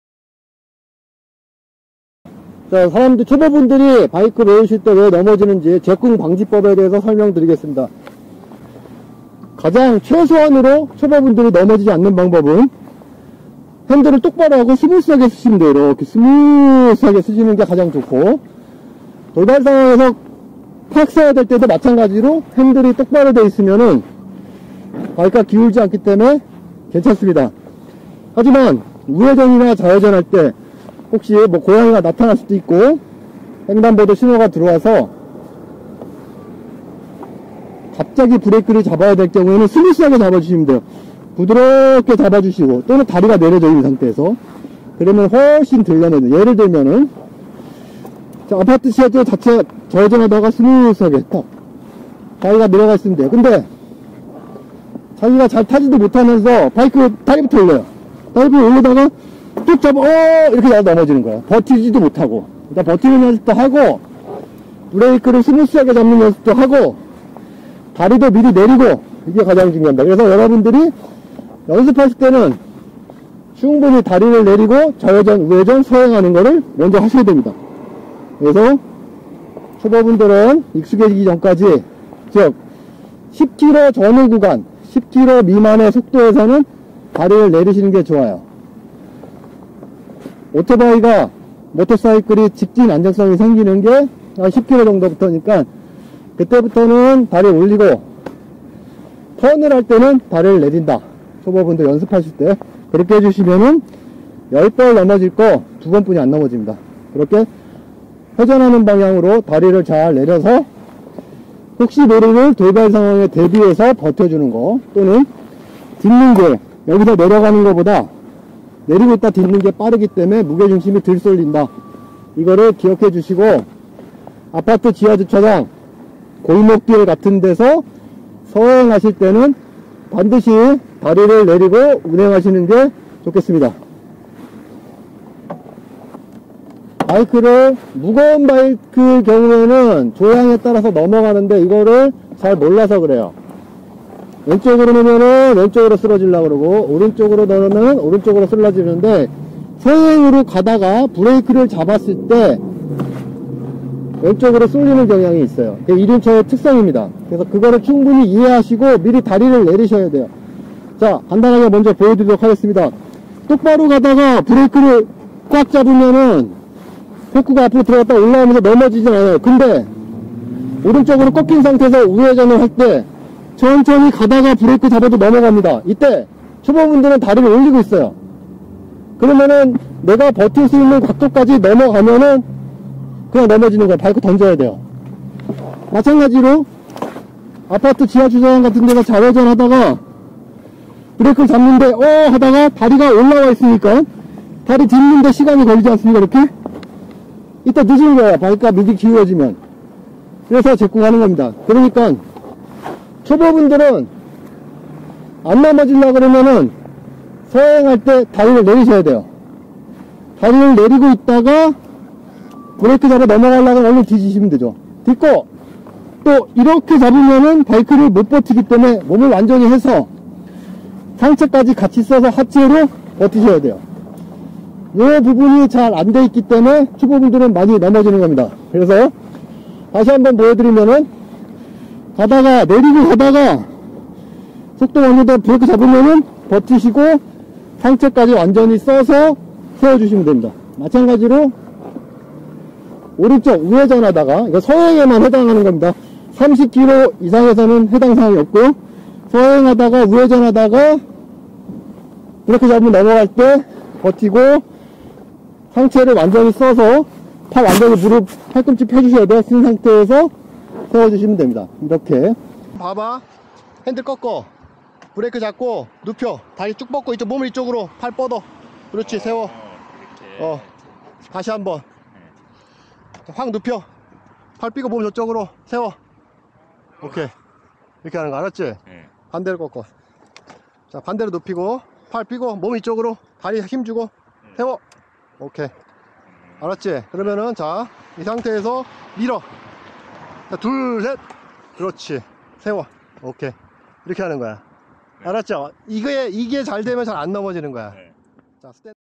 로드런너의 라이딩스쿨 #5 - 오토바이 입문자를 위한 제꿍 방지법
로드런너의 투휠라이딩 & 라이딩스쿨 운영하는 로드런너입니다.

모터사이클(오토바이)  입문자를 위한 강습 영상 다섯번째입니다.

제가 개인 강습 진행할 때 가장 중요하게 생각하는 부분입니다.

넘어지지 않는 것이 자신을 위해서 매우 중요하기

때문에 꼭 연습을 통해서 숙달할 필요가 있습니다.

실전 강습 영상도 후미 부분에 있으니 입문자 분들은 참고하시어 많이 연습하시길 바랍니다.

앞으로도 입문자를 위한 강습 영상을 계속 업로드 하도록 하겠습니다.

개인 강습 문의는 블로그를 참고하여 전화 상담 주시길 바랍니다.